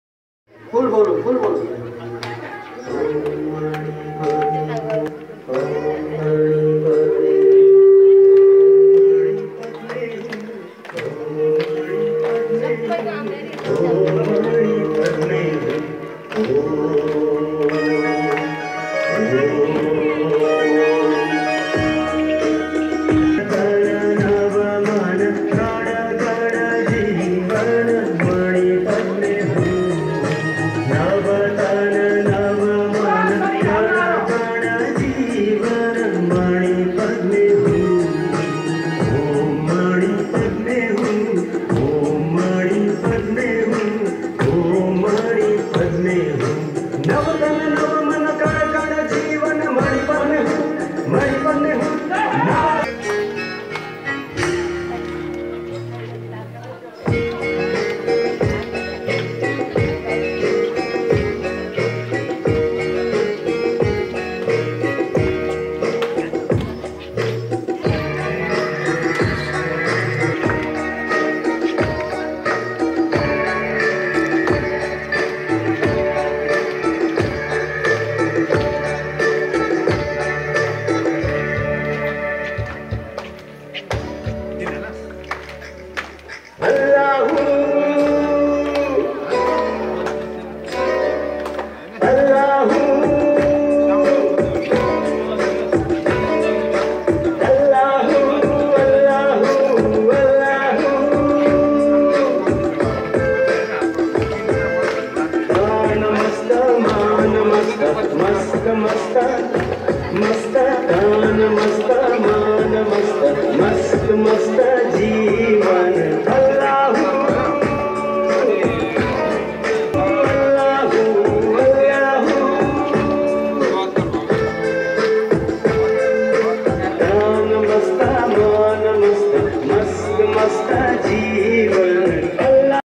नेपालमा योग|||yoga in nepal // laughing yoga
Yoga taught by bikasananda in galyang syangja